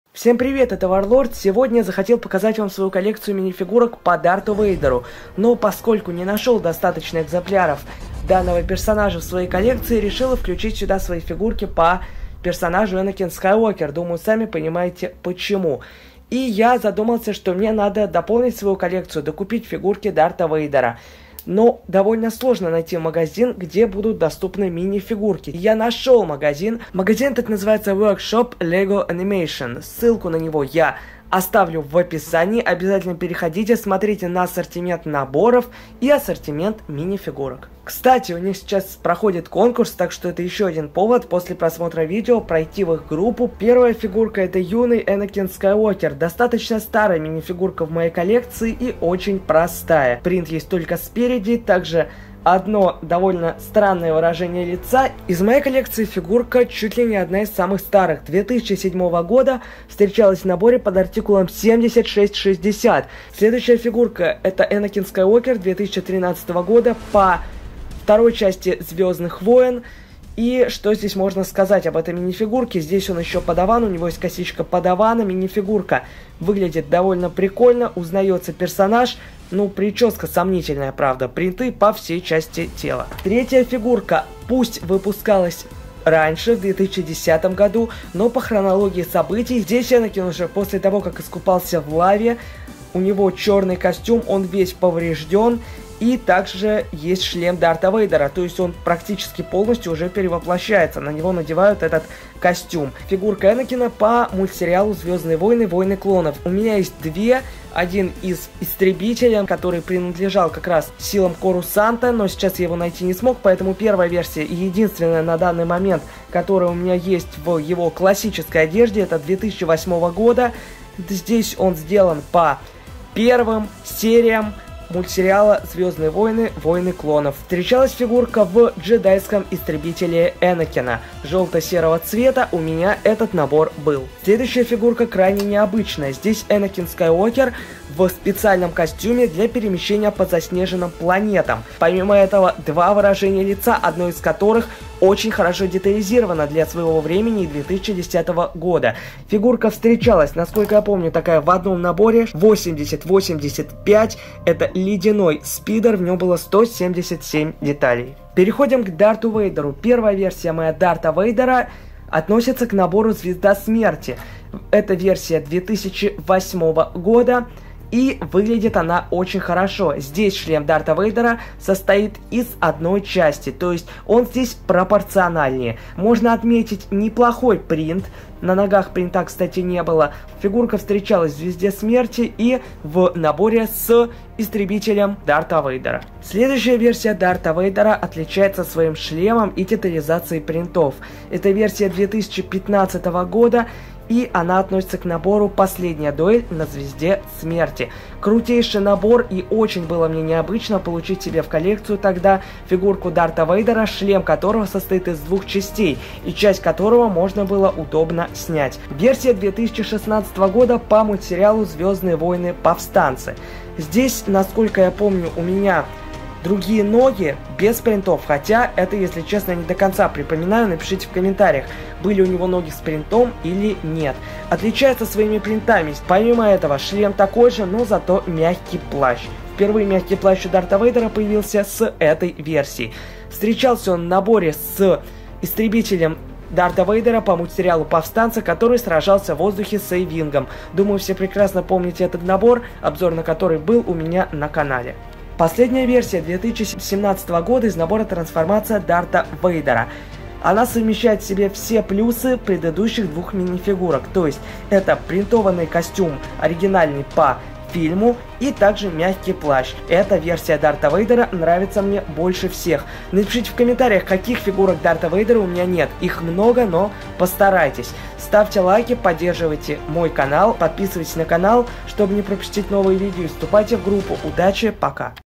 Дарт Вейдер LEGO Star Wars минифигурки вся моя коллекция Обзор
Лучшая группа по лего вконтакте Дарт Вейдер LEGO Star Wars минифигурки вся моя коллекция

Энакин Скайуокер, после перехода на тёмную сторону Силы Дарт Веедер — центральный персонаж Вселенной «Звёздных войн», главный антагонист киноэпопеи «Звёздные войны», на протяжении его становления в качестве проводника Силы, его переход на Тёмную сторону Силы и его итоговое искупление. Посмотрите все мои минифигурки Дарта Вейдера и Энакина Скайуокера за период с 2008 по 2017 годы.